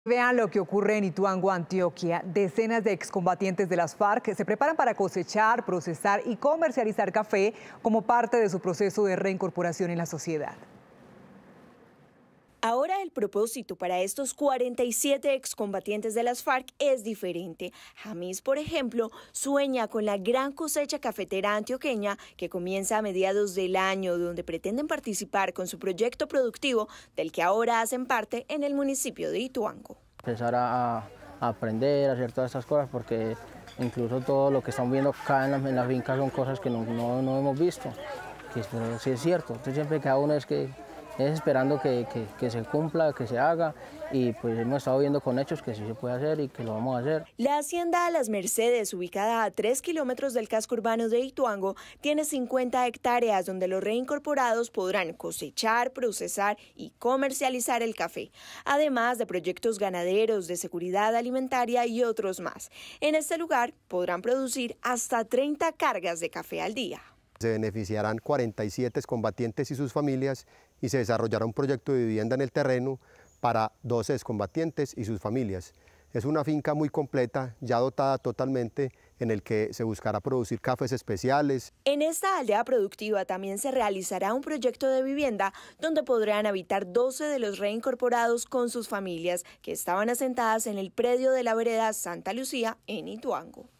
Proyecto cafetero, nueva ilusión de 47 excombatientes de las FARC en Ituango, Antioquia
Ahora el propósito para estos 47 excombatientes de las FARC es diferente. Jamis por ejemplo, sueña con la gran cosecha cafetera antioqueña que comienza a mediados del año donde pretenden participar con su proyecto productivo del que ahora hacen parte en el municipio de Ituango, norte de Antioquia.

“Empezar a aprender a hacer todas estas cosas porque incluso todo lo que estamos viendo aquí en la finca son cosas que no hemos visto entonces si es cierto a veces se queda uno esperando que se cumpla que se haga y hemos estado viendo con hechos que sí se puede hacer y que lo vamos a hacer”, expresó el excombatiente de las FARC Jamis Valle.

La hacienda Las Mercedes, ubicada a 3 kilómetros del casco urbano de Ituango, tiene 50 hectáreas donde los reincorporados podrán cosechar, procesar y comercializar el café, además, de proyectos ganaderos, de seguridad alimentaria. En este lugar podrán producir hasta 30 cargas de café al día.